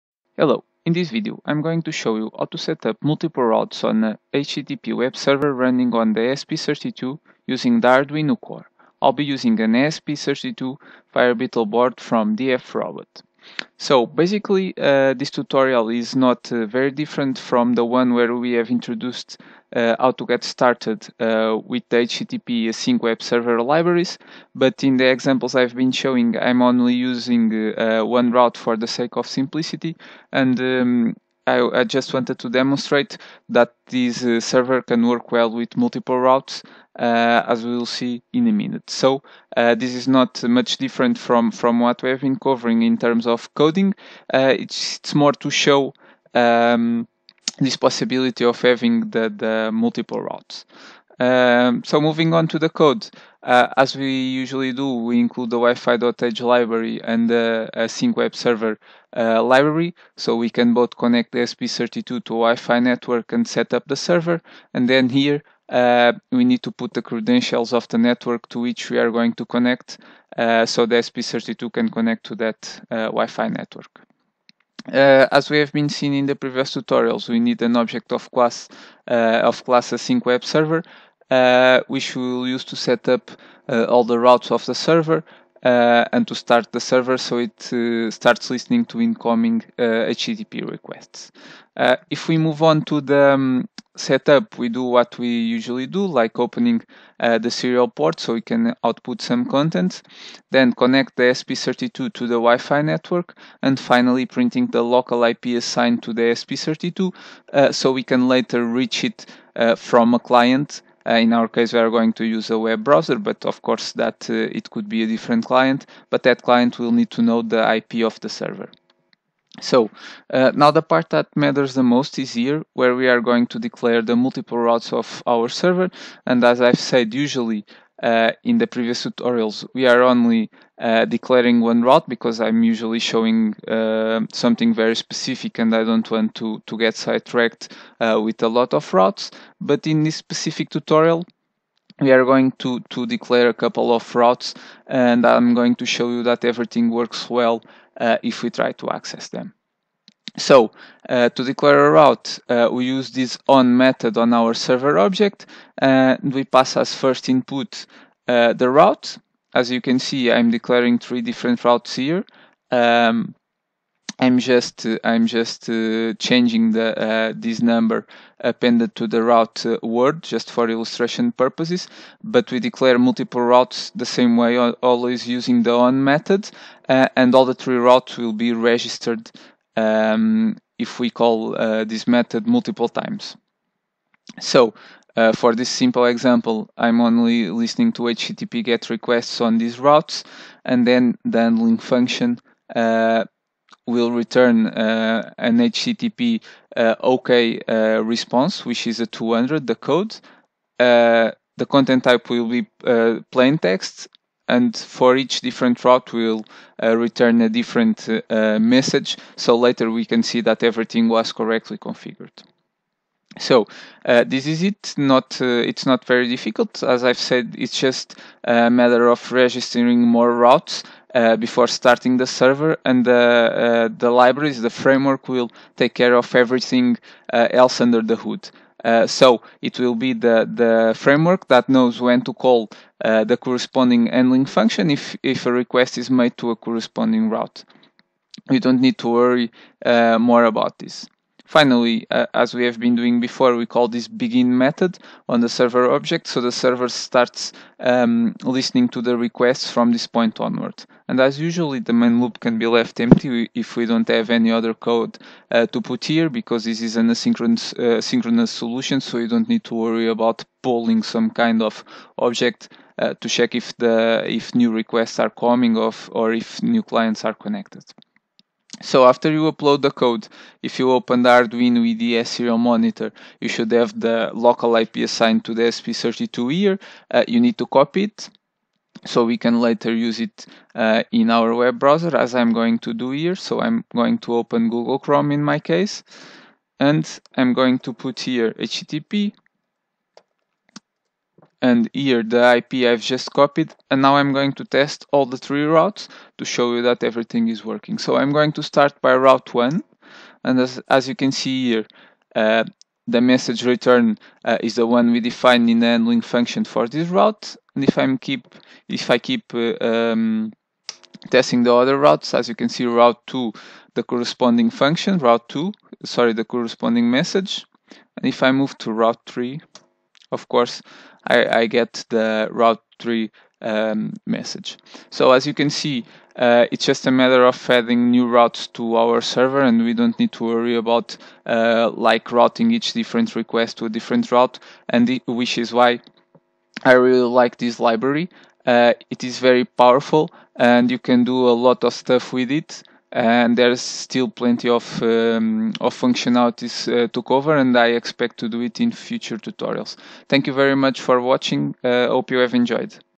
ESP32 Arduino HTTP web server: multiple routes
This video shows how to setup multiple routes on a HTTP web server, running on the ESP32 and using the Arduino core.